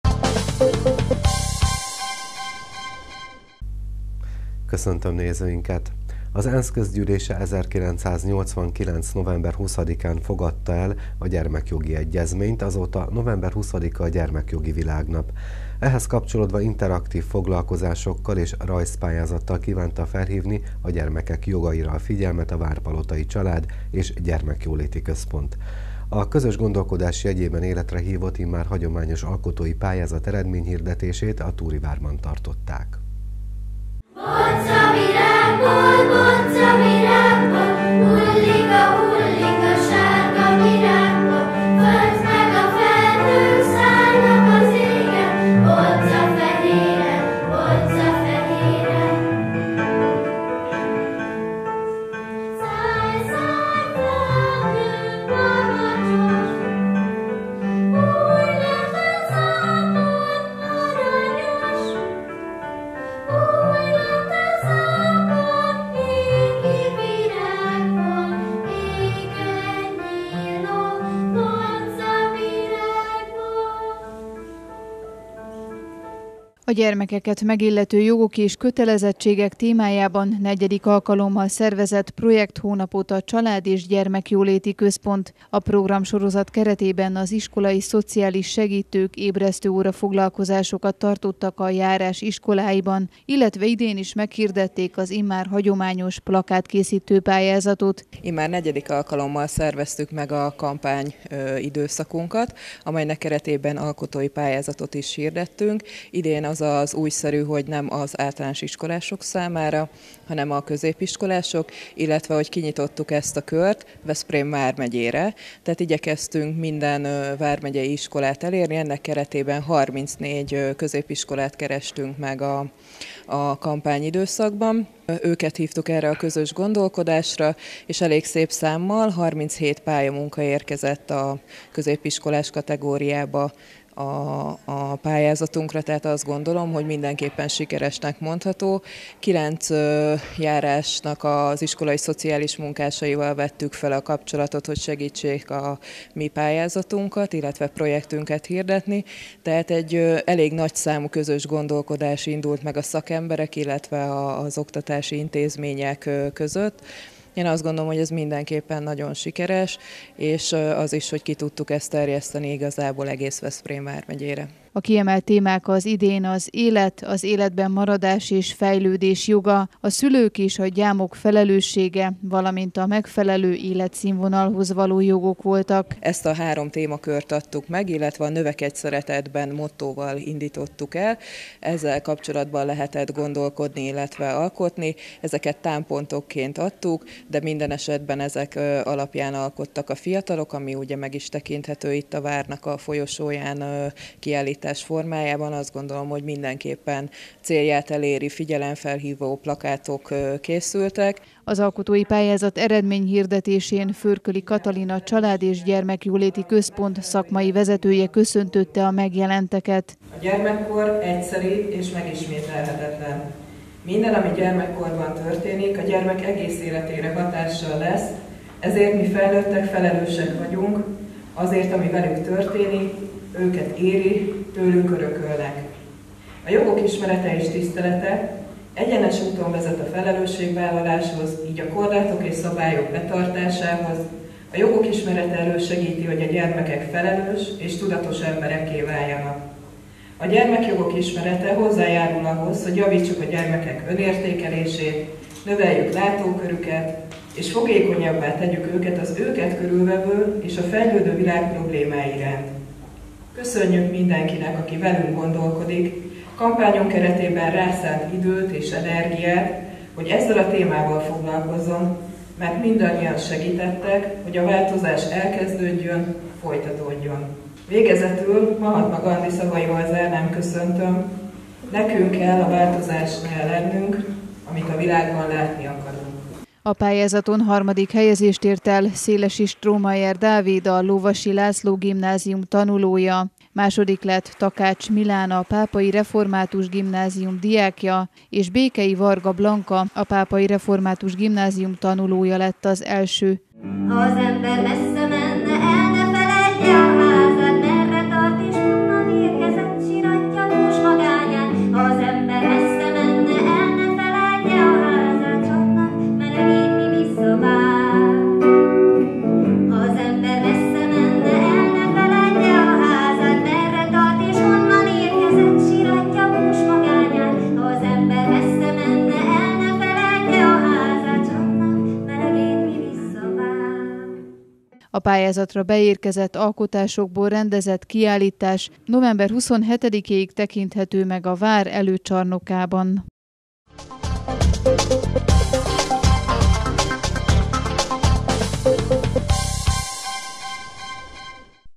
"Növekedj szeretetben" - rajzpályázat díjkiosztó a Thury várban
Az ENSZ Közgyűlése 1989. november 20-án fogadta el a Gyermekjogi Egyezményt, azóta november 20-a a Gyermekjogi Világnap. Ehhez kapcsolódva interaktív foglalkozásokkal és rajzpályázattal kívánta felhívni a gyermekek jogaira a figyelmet a várpalotai Család- és Gyermekjóléti Központ. A közös gondolkodás jegyében életre hívott, immár hagyományos alkotói pályázat eredményhirdetését a  Thury-várban tartották.